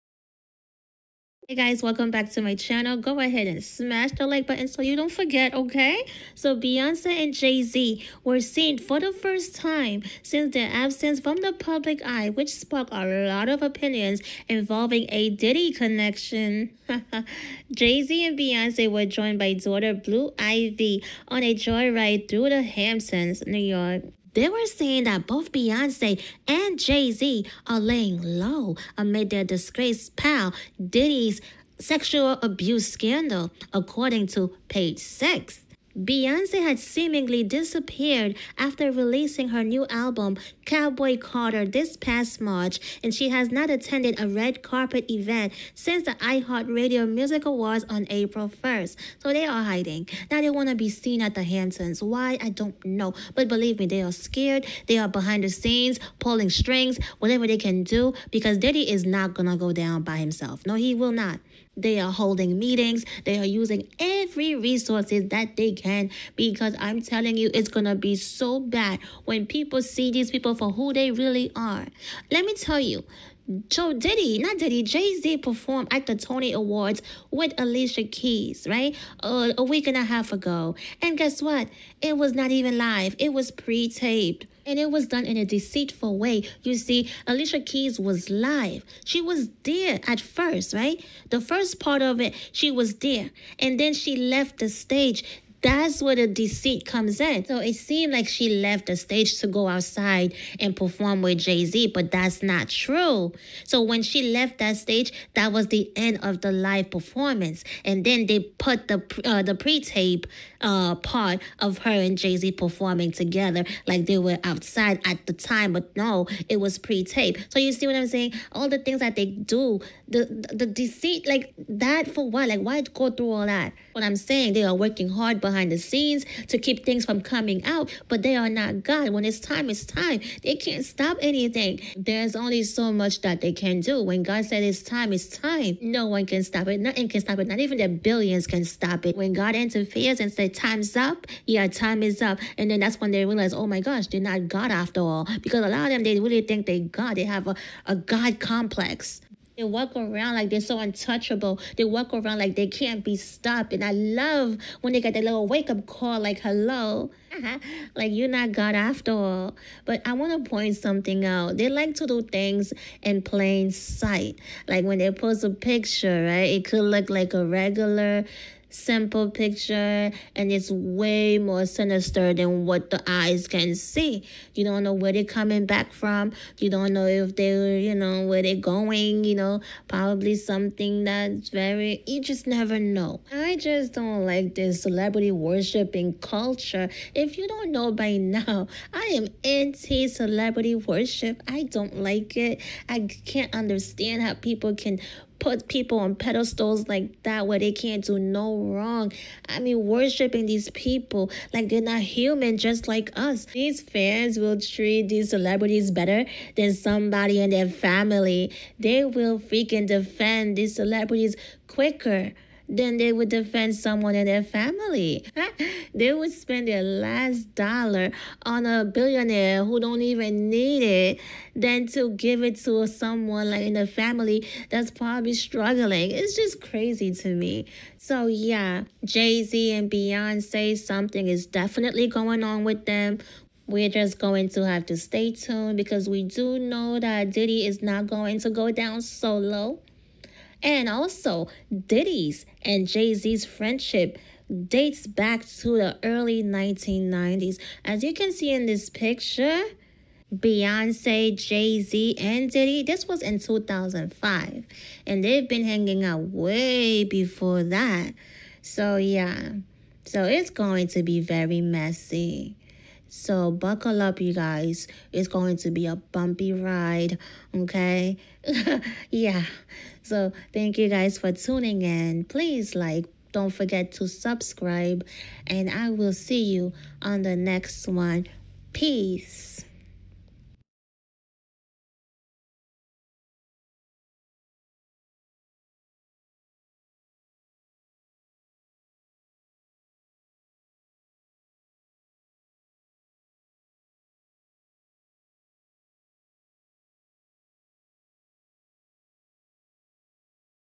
Beyonce & Jay-Z spotted for the first time after 3-months in hiding
Beyonce and Jay-Z were seen for the first time after 3-months 'laying-low' amid Diddy's sexual abuse scandal. Where were they spotted?